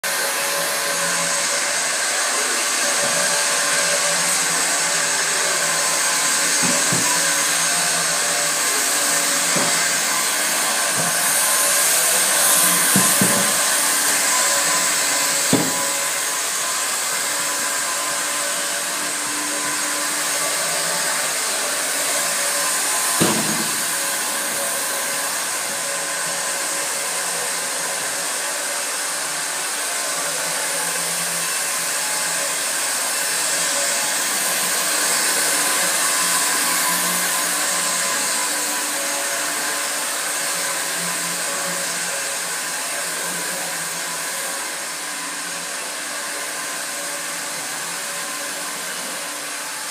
i mop in Action
Look how fast and manoeuvrable the imop is, could you do this with any other scrubber dryer?